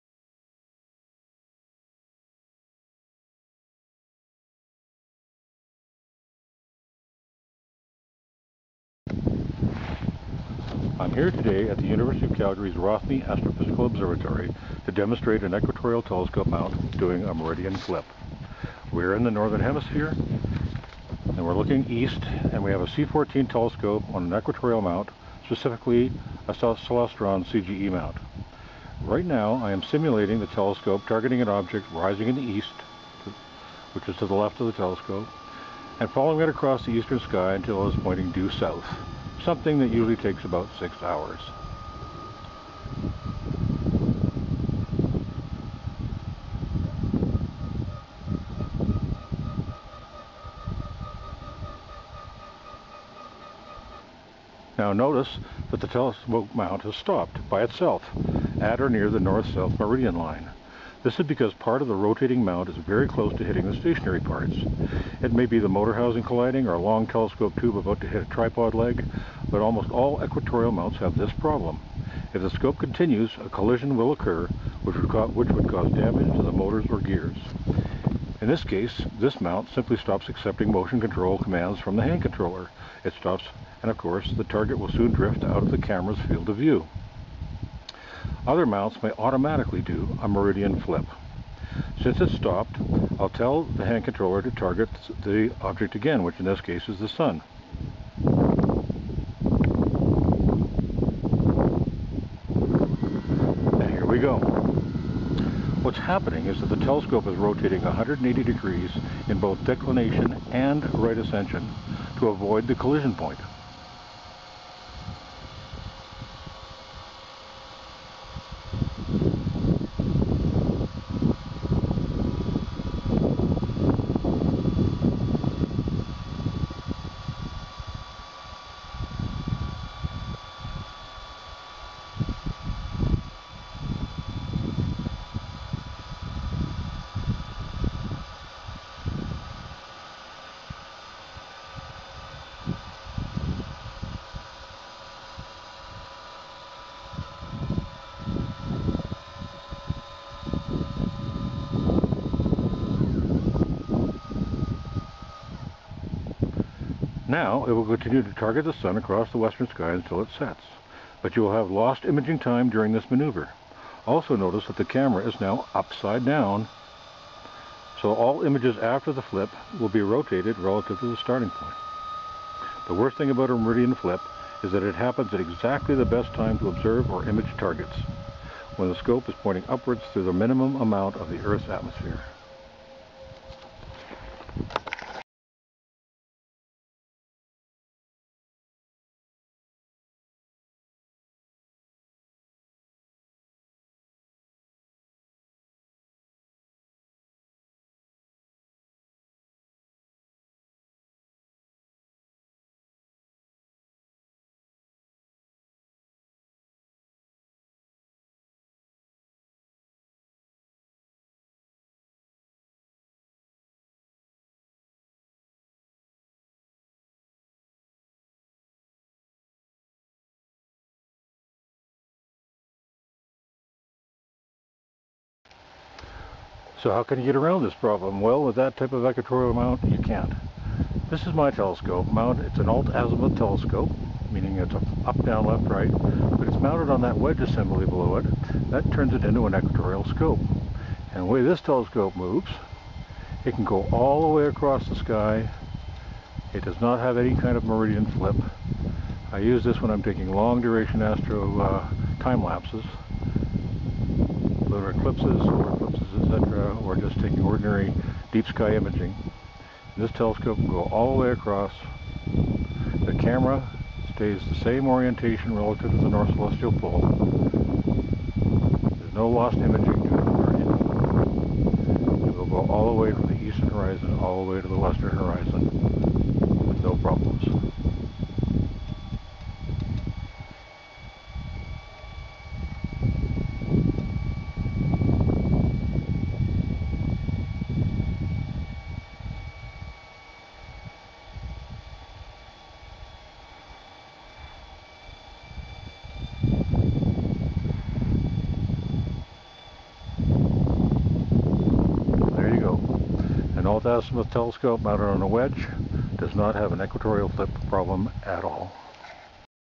Equatorial Mount Meridian Flip Dec 6 2017 by Larry McNish
A demonstration and explanation of the Meridian Flip problem with  telescopes on a German Equatorial Mount. Includes notes, cautions, and a demo of a telescope mount that does not have this problem.